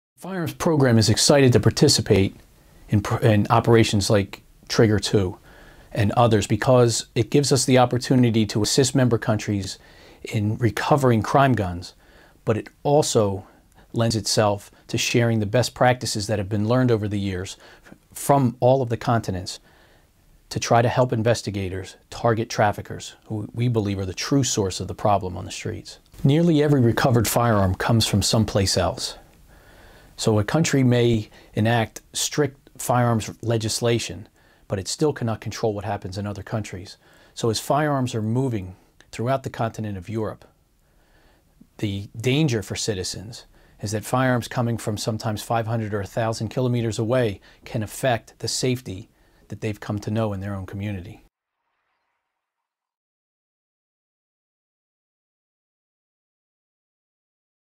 John Hageman, INTERPOL Firearms Programme, on Operation Trigger II
An operation coordinated by INTERPOL against illicit firearms trafficking in Europe has seen 149 arrests and the seizure of firearms, ammunition and explosives.

“Operations such as Trigger II also give us an opportunity to assist member countries share best practices to help investigators target traffickers whom we believe are the true source of the problem on our streets,” said Mr Hageman.